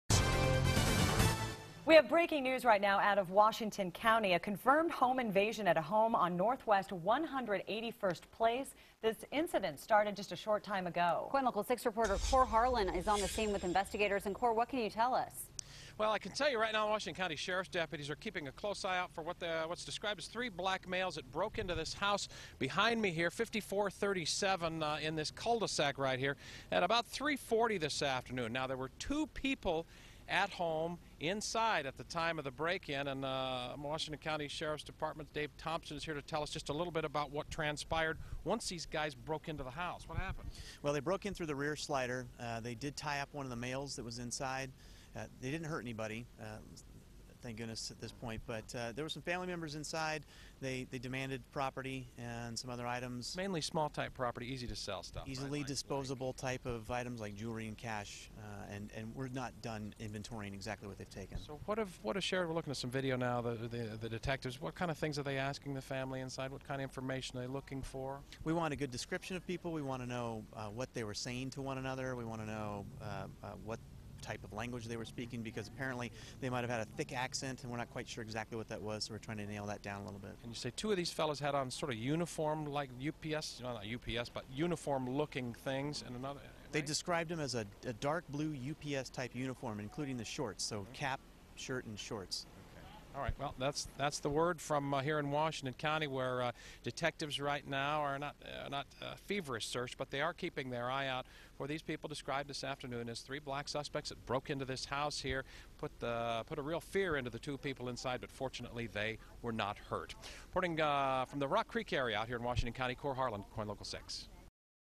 Three suspects sought in Rockcreek home invasion
Authorities are on the scene of a possible home invasion in the Rockcreek area of Northwest Portland.